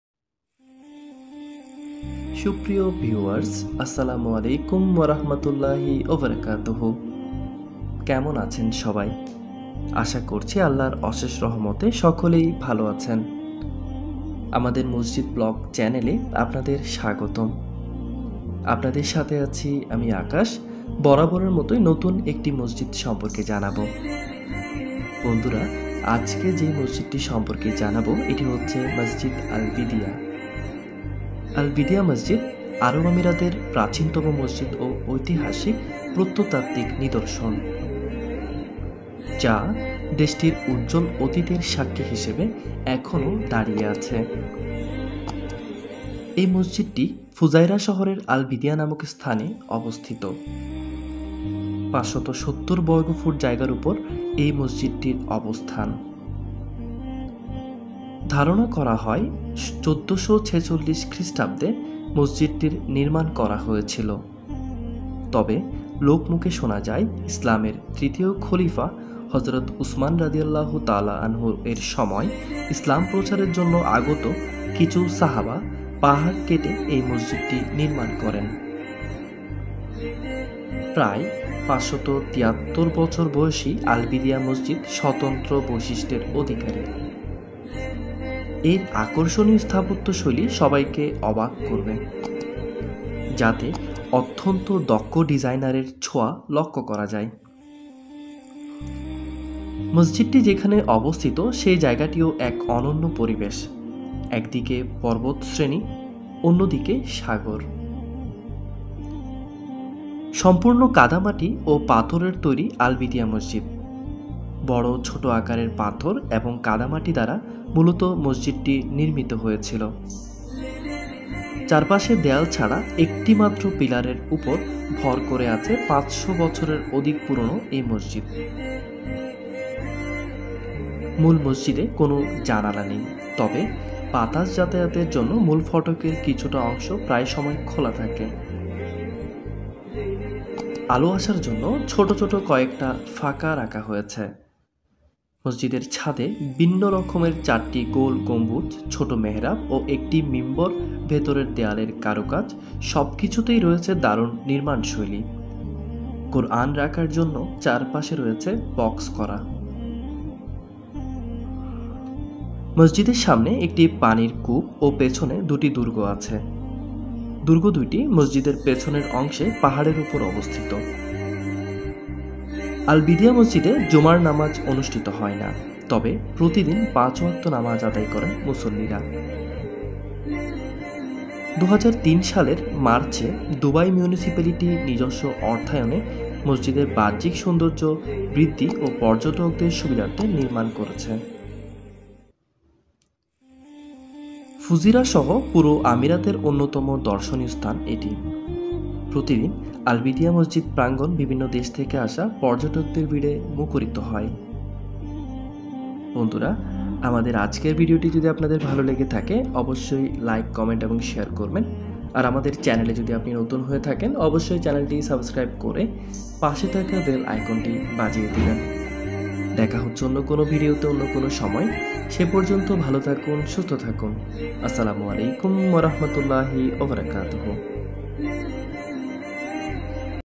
৫০০ বছরের পুরনো আল বিদিয়া মাটির মসজিদ, আরব আমিরাত, ফুজাইরাহ| United Arab Emirates, Fujairah
৫০০ বছরের পুরনো আল বিদিয়া মাটির মসজিদ, আরব আমিরাত, ফুজাইরাহ| United Arab Emirates, Fujairah
আল বিদিয়া মসজিদ আরব আমিরাতের প্রাচীনতম একটি মাটির মসজিদ। ৫০০ বছরের পুরনো এই মসজিদটি দেশের উজ্জ্বল অতীতের সাক্ষী হিসেবে এখনো দাঁড়িয়ে আছে। ফুজাইরা শহরের আল বিদিয়া নামক স্থানে মসজিদটি অবস্থিত।

Most popular humming islamic music no copyright